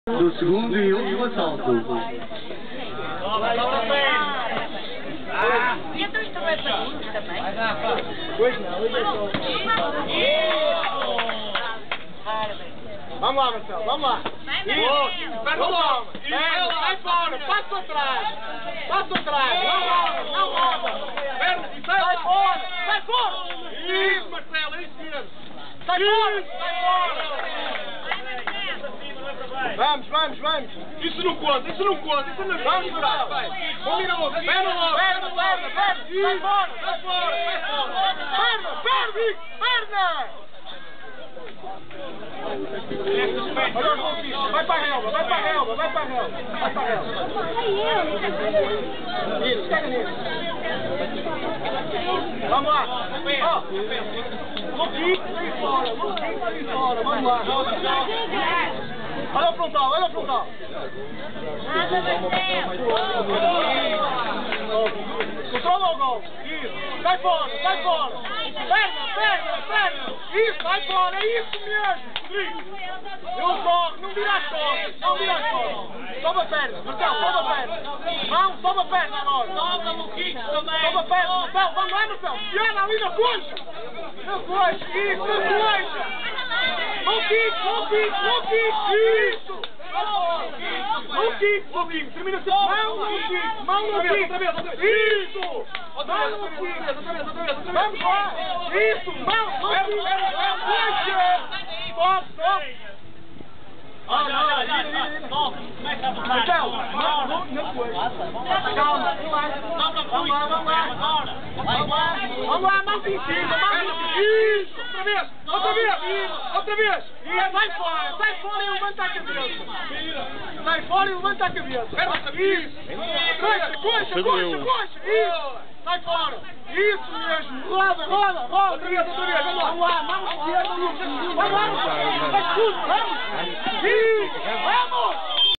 3º vídeo - 1º Torneio de Light Kick / Contact
1º Torneio de Light Kick / Contact, organizado pela Associação Desportiva de Penafiel, no Parque da Cidade em Penafiel.
Um agradável convívio entre os atletas desta Associação e atletas convidados, durante cerca de três horas.